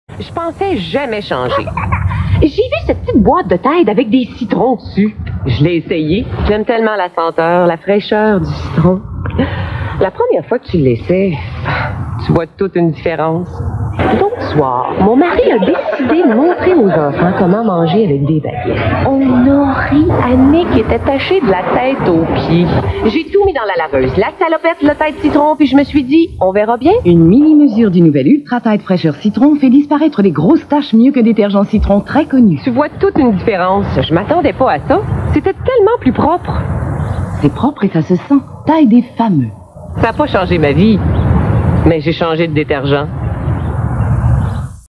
Ultra Tide avec Fraîcheur Citron 1992 (CAN/F)
Kanadischer Fernsehwerbespot in französischer Sprache für das Waschmittel Ultra Tide avec Fraîcheur Citron aus dem Jahr 1992. @1978WAM

Publicité télévisée canadienne en français pour la lessive Ultra Tide avec Fraîcheur Citron de 1992. @1978WAM